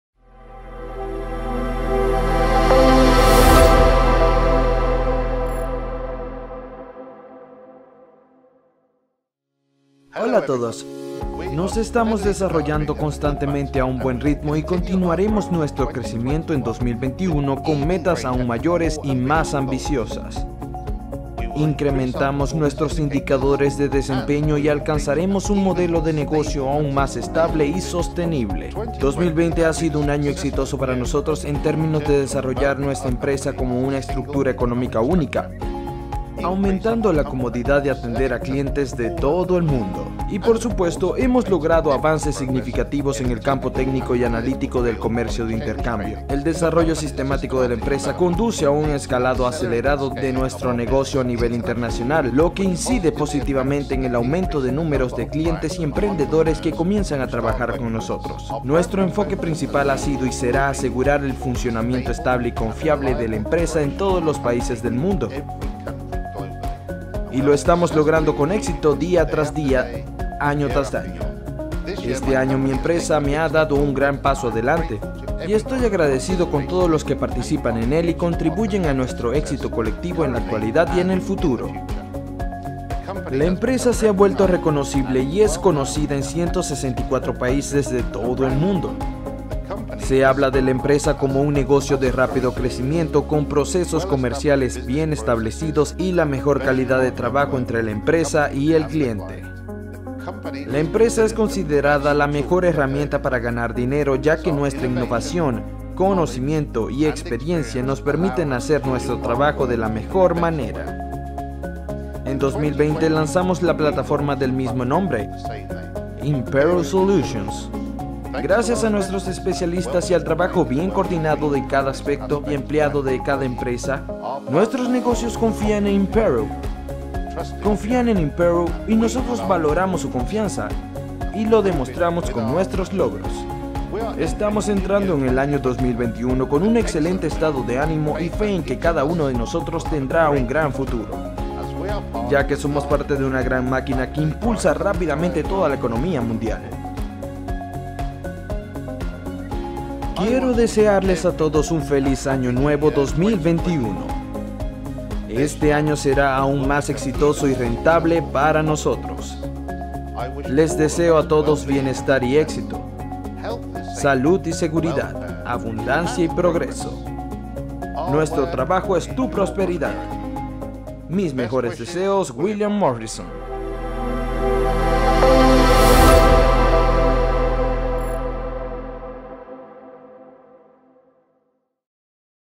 Saludos de año nuevo del CEO de Impero.solutions (ESP)
En este video, el CEO de Impero.solutions felicita a todos los inversores de la compañía por las vacaciones de Año Nuevo.